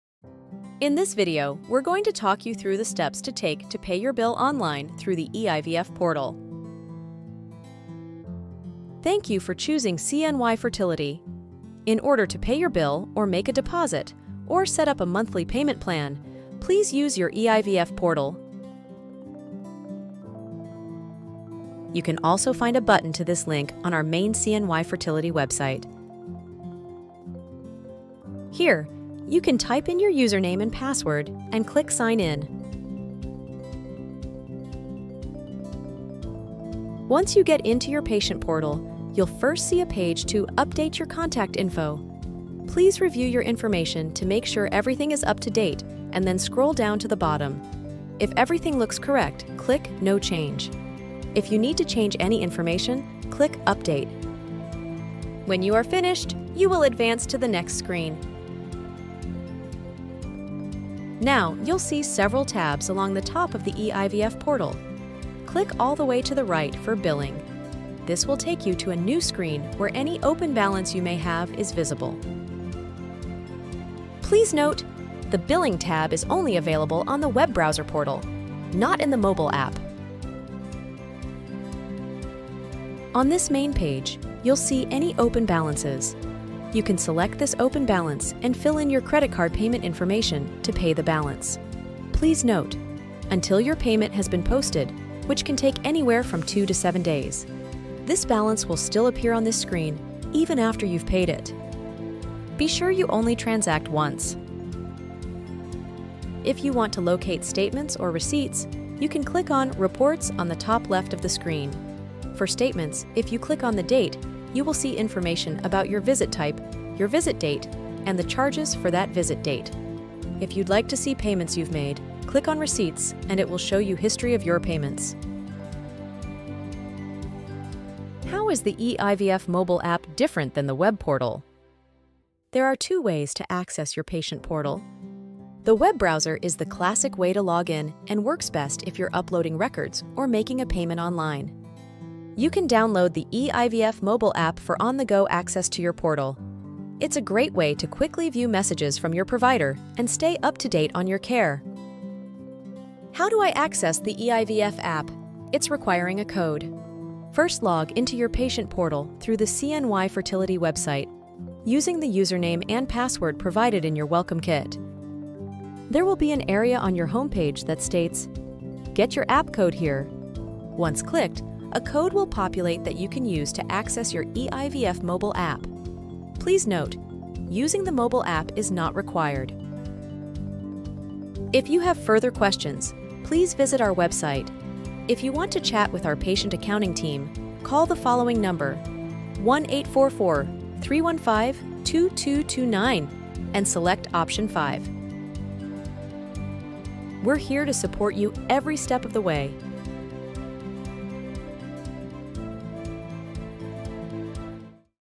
How do I access CNY Fertility's Patient Portal and the eIVF app? How do I pay my bill online?
00:00 - Introduction
00:08 - Signing Into Your eIVF Portal
00:35 - Once Signed In: Update Info
00:59 - How To: Pay Your Bill
01:45 - How To: Locate Statements or Receipts
02:07 - eIVF: Web Browser vs. Mobile App
02:33 - Accessing Mobile App
03:02 - Questions?